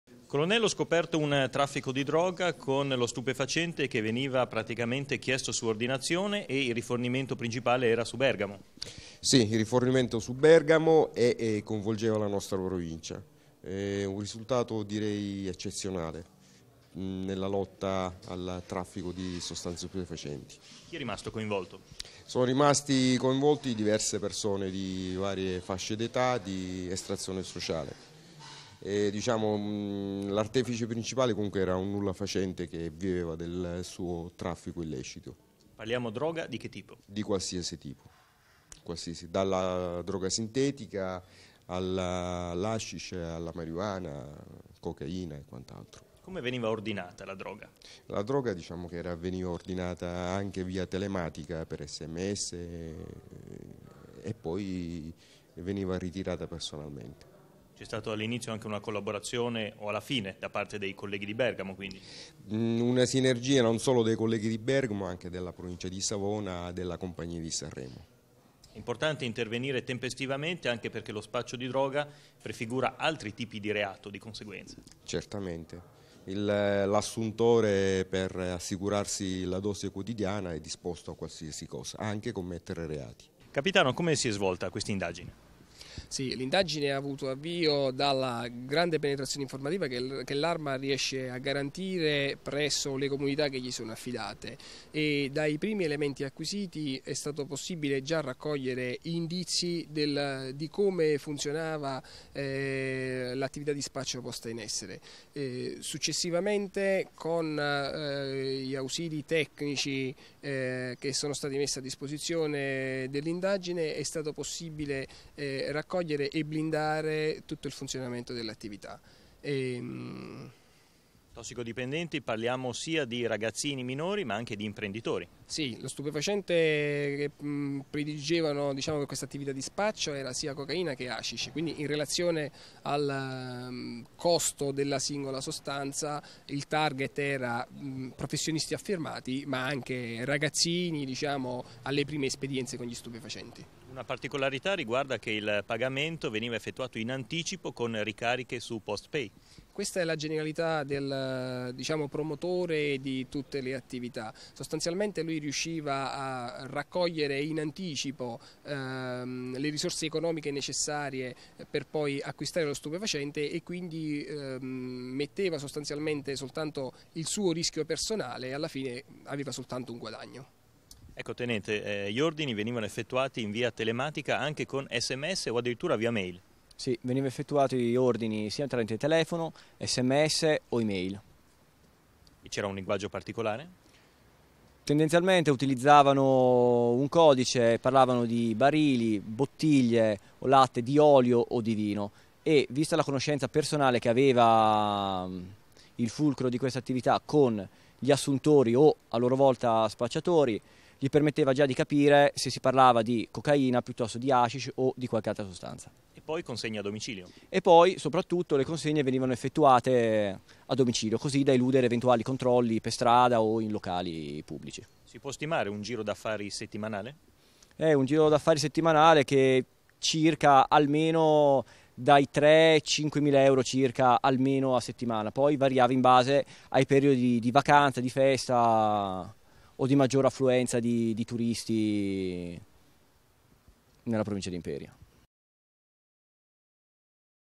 OPERAZIONE ANTI-DROGA DEI CARABINIERI DI IMPERIA
Importante operazione antidroga da parte dei carabinieri di Imperia che hanno scoperto un traffico di stupefacenti tra Bergamo e il ponente ligure. La droga, cocaina, marijuana e sostanze sintetiche, veniva ordinata tramite sms o anche via mail utilizzando messaggi in codice. I pagamenti venivano effettuati in anticipo tramite ricariche su carte Post Pay